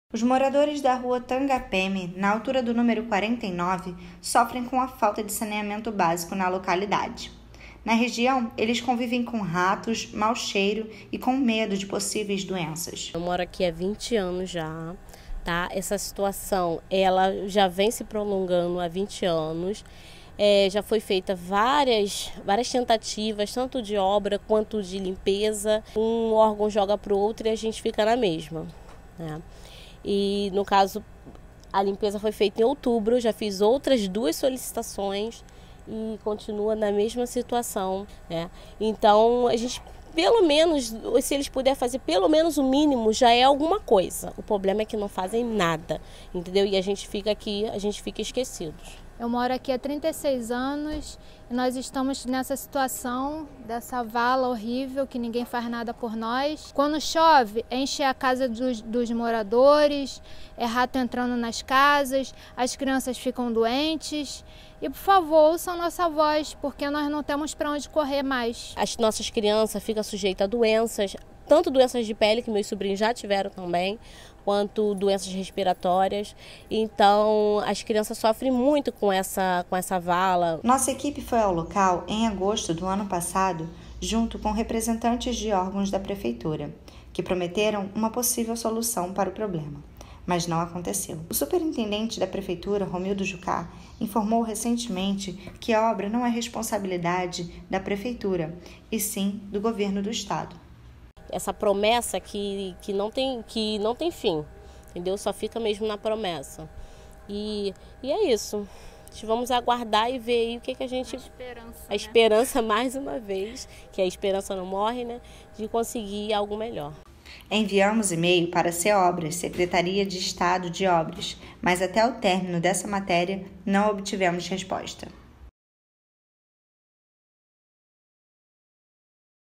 Moradores do Complexo do Alemão denunciam falta de saneamento básico na região
Nossa equipe esteve novamente na Fazendinha, para mostrar que as condições dos moradores da rua Tangapeme, não melhoraram.
A falta de saneamento básico na região é um problema de décadas. Após um novo pedido dos moradores, fomos até o local para registar que nada foi resolvido, a vala continua escorrendo a céu aberto.
Entramos em contato com a SEOBRAS, para solucionar o caso, mas ainda não tivemos retorno. Continuaremos acompanhando o caso.